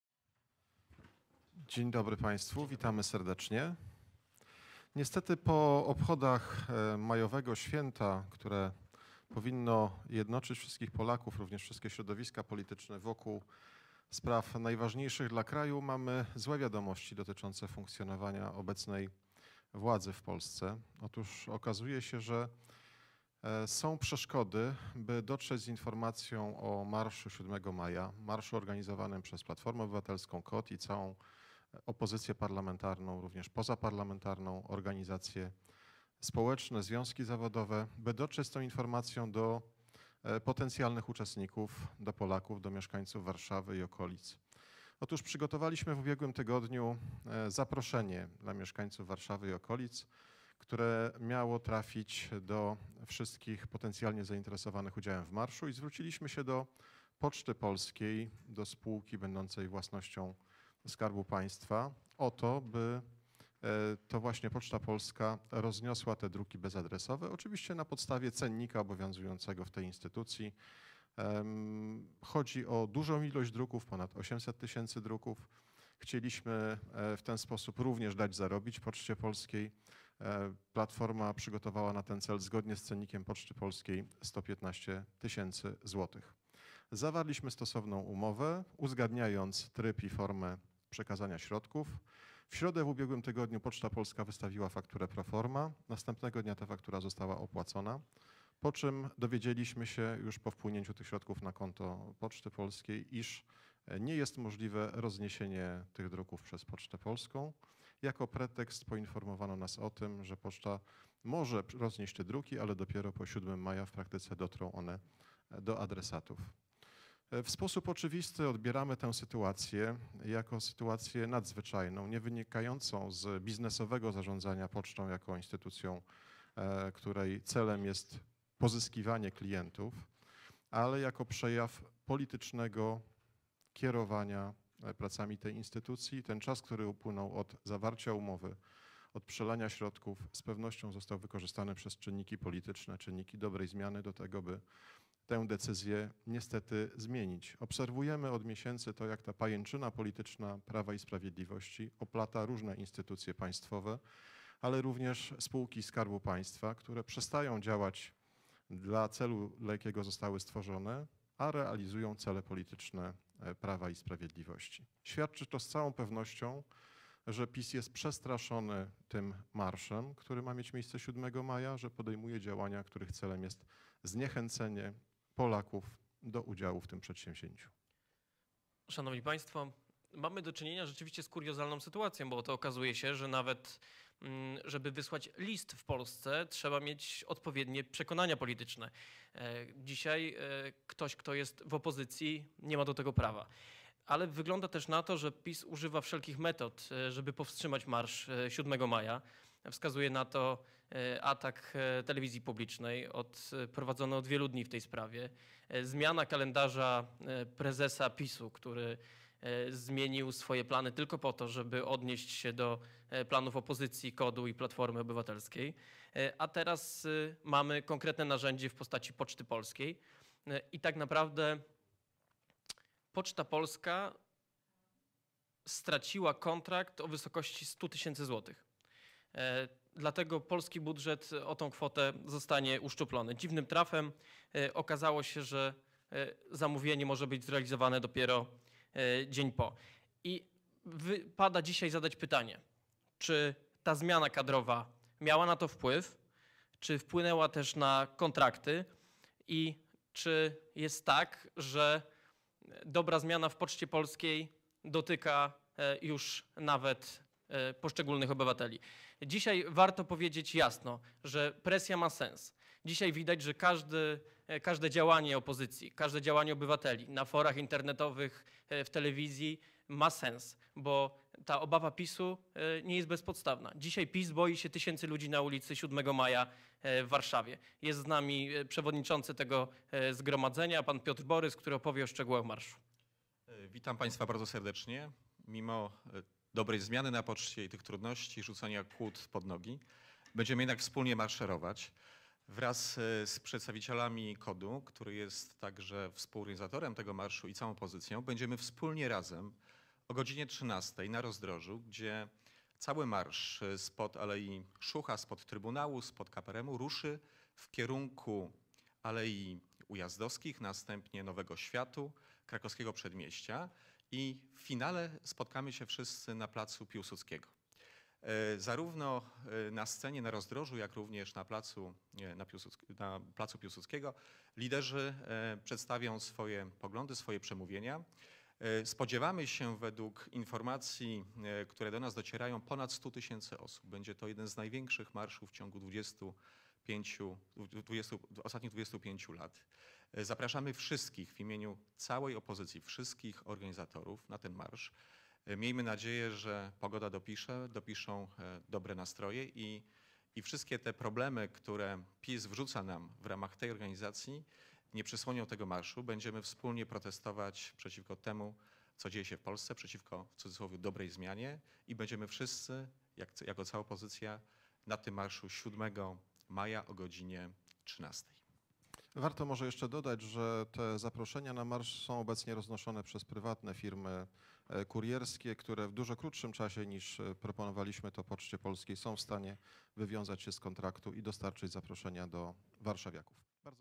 Państwo PiS blokuje Marsz 7 maja
Platforma Obywatelska natrafiła na przeszkody by dotrzeć do społeczeństwa z informacją o marszu organizowanym 7 maja w Warszawie. Jan Grabiec, rzecznik prasowy Platformy Obywatelskiej, podczas konferencji prasowej w Sejmie zwrócił uwagę, że PiS obawia się wyjścia Polaków na ulice Warszawy, dlatego chce ich zniechęcić do udziału w marszu wykorzystując do tego Pocztę Polską.